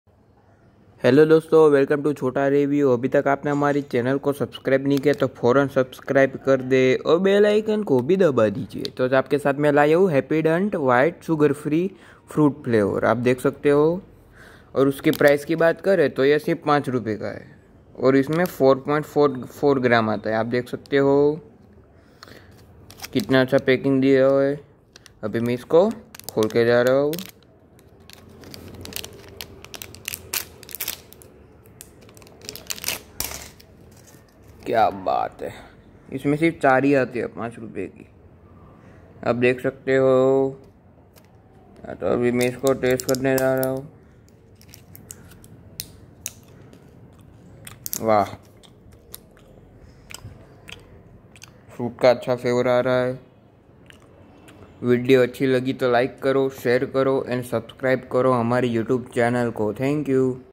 HAPPYDENT white FRUITS FLAVOUR sugarfree with XYLITOL full review and taste
Hi
How are you
I am fine

Aaj me aapke liya laya hu HAPPYDENT white FRUITS FLAVOUR sugarfree with XYLITOL chungum ka full review price net weight and taste janege